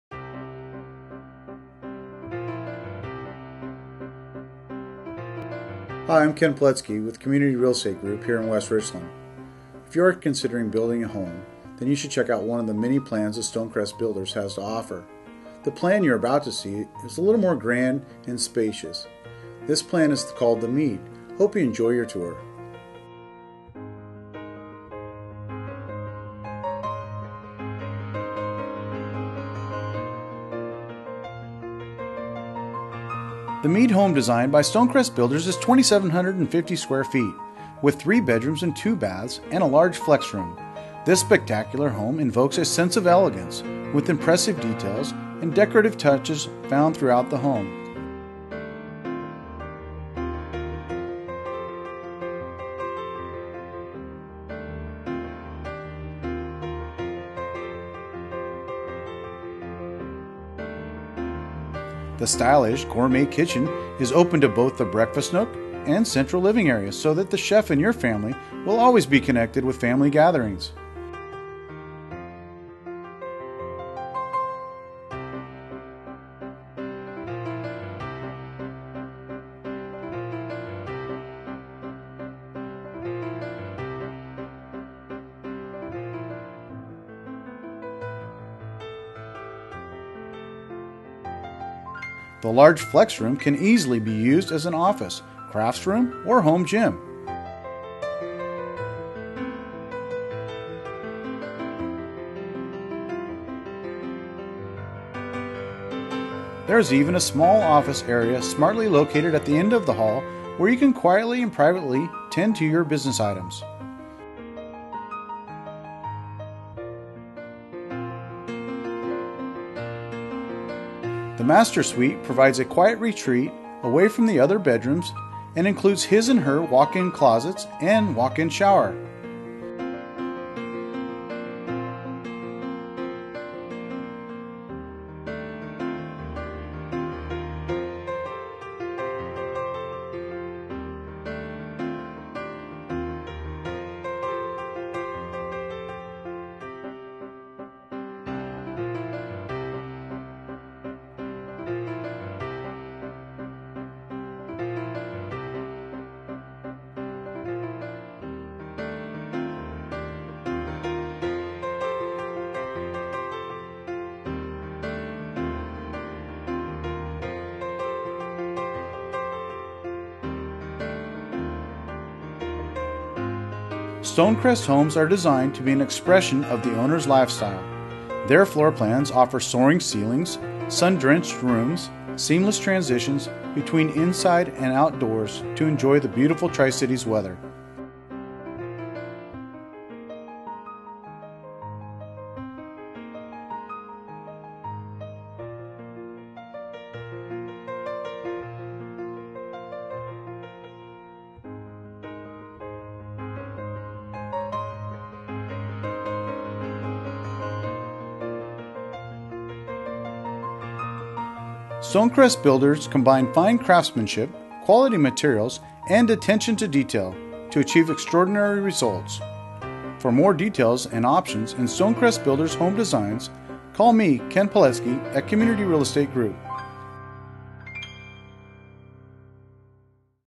Ken Poletski introduces StoneCrest Builder's plan called The Meade
Published 7/24/2013
By Ken Poletski, Community Real Estate Group

StoneCrest Builder's presents another winning floor plan called "The Meade".  This plan is a beautiful design which meets all the needs of home buyers today.  Open spacious floor plan with incredible finish accents this mid sized 2700 sqft home.  A super sized master suite has extra large bedroom and spa like bath.  Both other bedrooms are larger than your finding in other homes of similar prices.  The custom hidden office space can double as wine cave, craft room, or children's homework room.  The large media can be used for a multi purpose space as a office, study, work out room or formal dining room.  Ask us how to modify our plans to meet your needs.  StoneCrest Builders has so many plans with the custom features you see in this video as their standard finish.  If you are thinking of building a new home come check this out in West Richland, Wa.